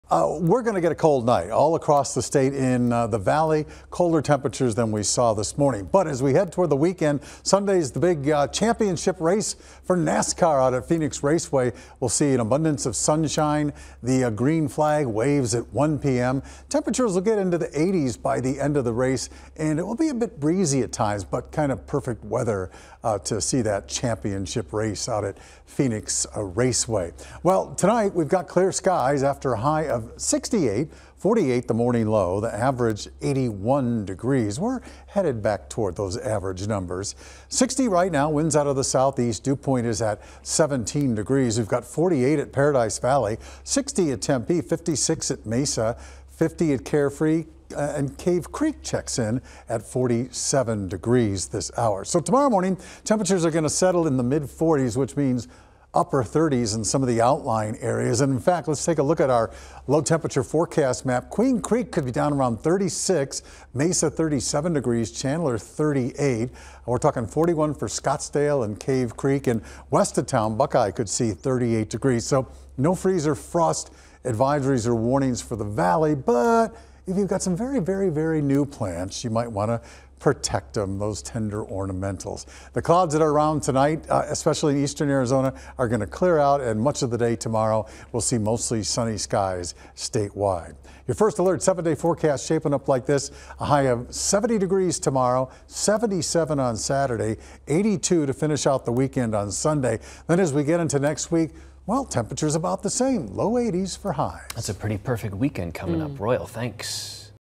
Arizona will see another cold night before a warmer weekend
AZFamily First Alert Weather 9 p.m. Update for Thursday, 11/07/24